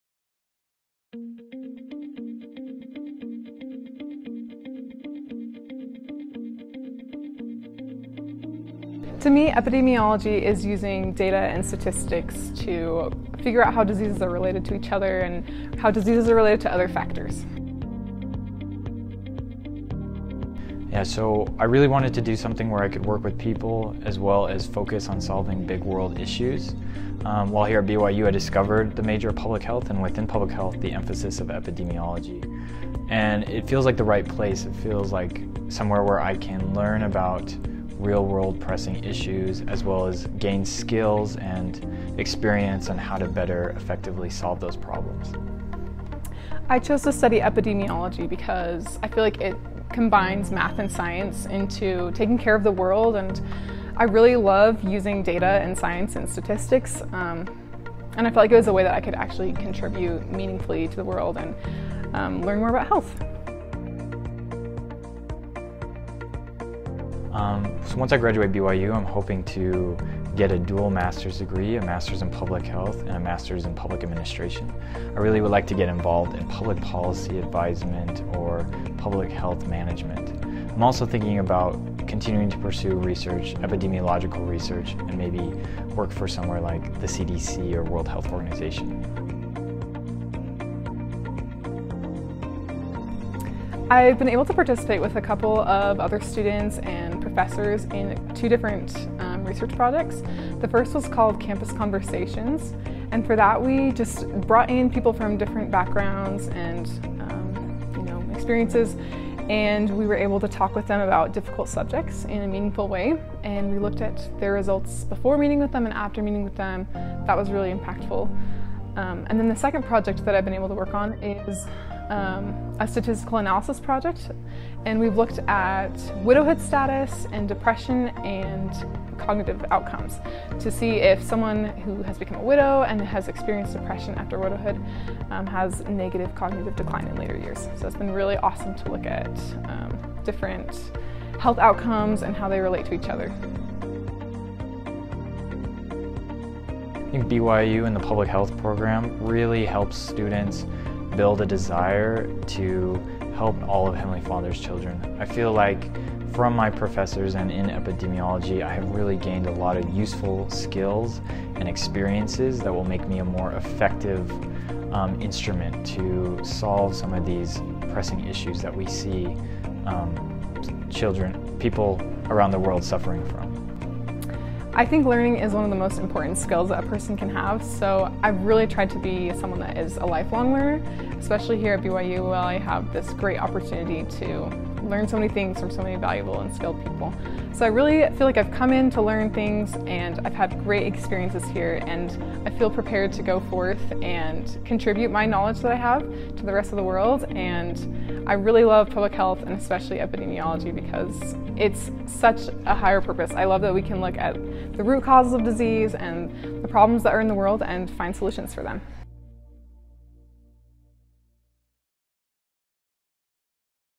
What is Epidemiology?
BYU Epidemiology students talk about their experience in the major.

Epidemiology is one of four emphases in the public health major at Brigham Young University.